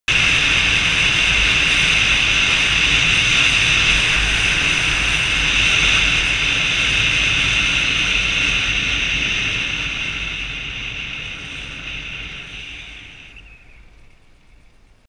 40mph
coming down from hilltop park after biking to the top of black mtn for sunset.